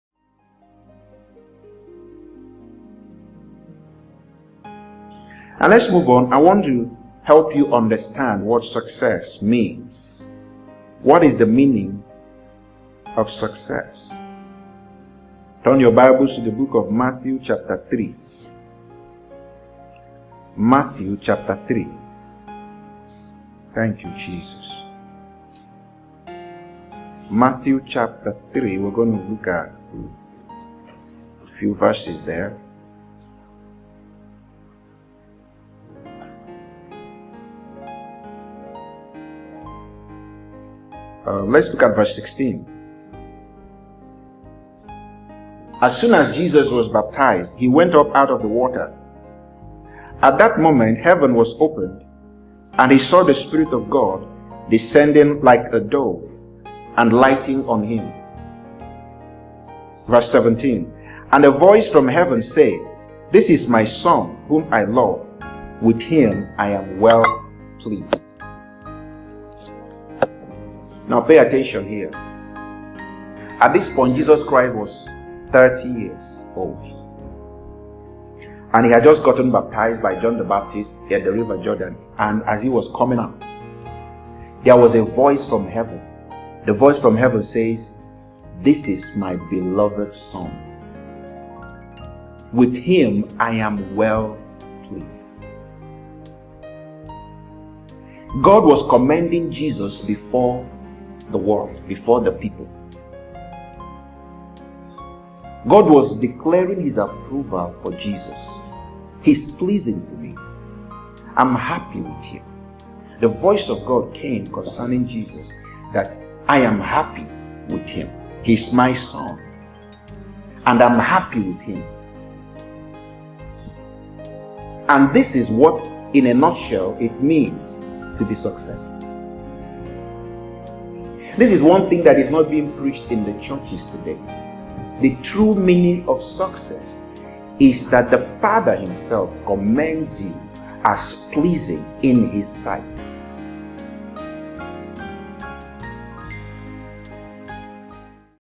The True Meaning of Success
The true meaning of success is that the Father commends you as pleasing in His sight. Any other thing you achieve apart from this is just another mound of dung.